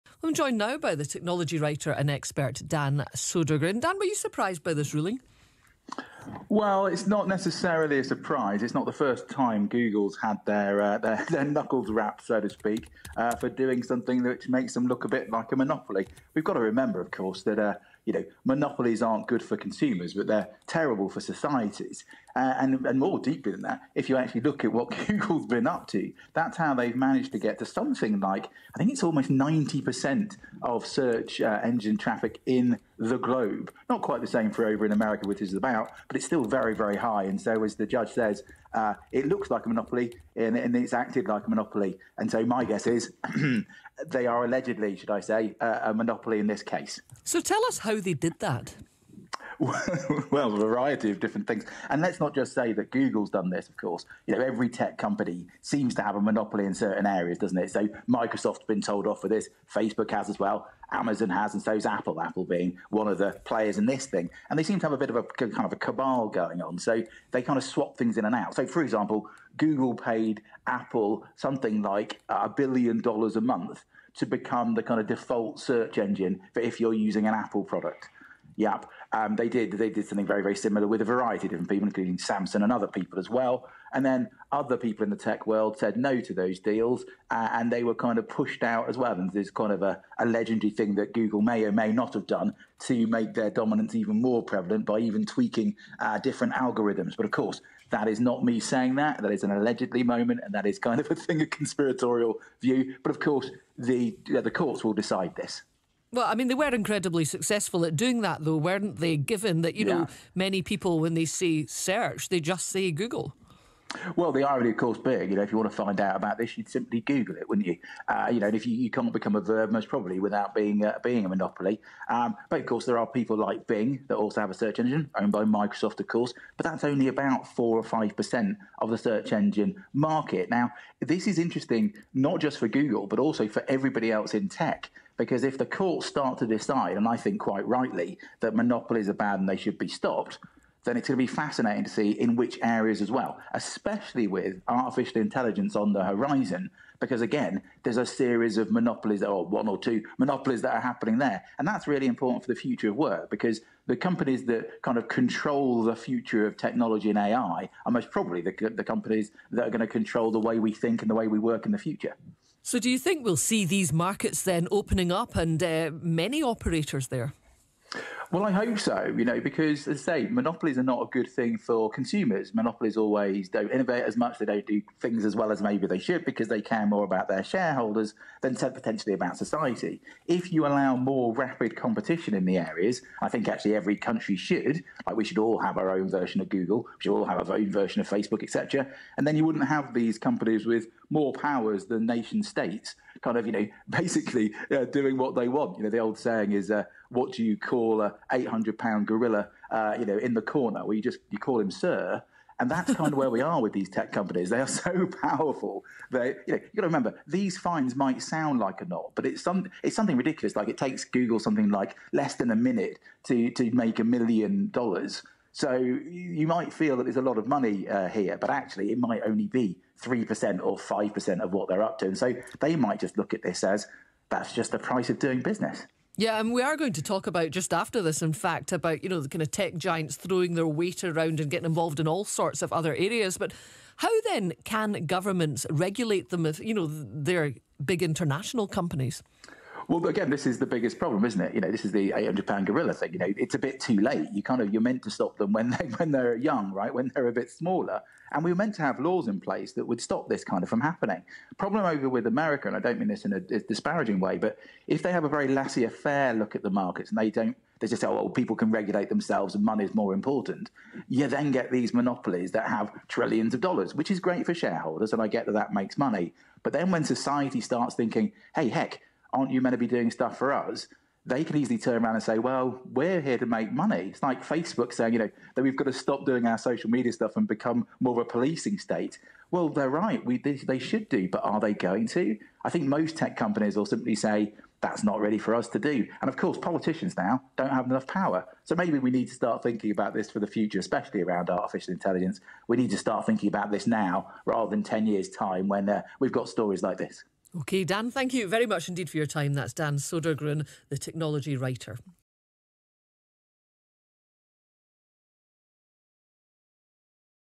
Dan Sodergren on BBC Radio talking about the implications of the Google AntiTrust Ruling In America.
It's a long one. But an important radio discussion about @Google and their antitrust case and being outed as a monopoly...

But why are tech giants, when they get too big and act like monopolies, bad for the world?

And what can we do about them?

And how does this change the future?

For not only AI but the futureofwork too.

In the FifthIndustrialRevolution.

Thanks for having me on @BBCRadioScot and calling me a technology writer. My mum will be pleased...

I guess I did write about ArtificialIntelligence and technology in the FifthIndustrialRevolution book available as a digital download on Gumroad

Just never considered my title as a tech writer... Sounds dead posh.